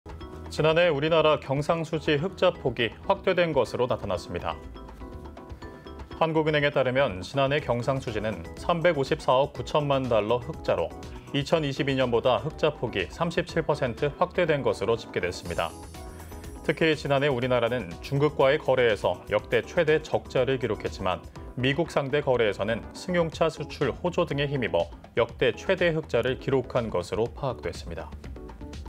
[소셜] 경제ㅣ경상수지 흑자 폭 확대, 중국은 최대 적자 미국은 최대 흑자 (이한승)ㅣCTS뉴스
지난해 우리나라 경상수지 흑자 폭이 확대된 것으로 나타났습니다.

한국은행에 따르면 지난해 경상수지는 354억 9,000만 달러 흑자로 2022년보다 흑자 폭이 37% 확대된 것으로 집계됐습니다.

특히 지난해 우리나라는 중국과의 거래에서 역대 최대 적자를 기록했지만 미국 상대 거래에서는 승용차 수출 호조 등에 힘입어 역대 최대 흑자를 기록한 것으로 파악됐습니다.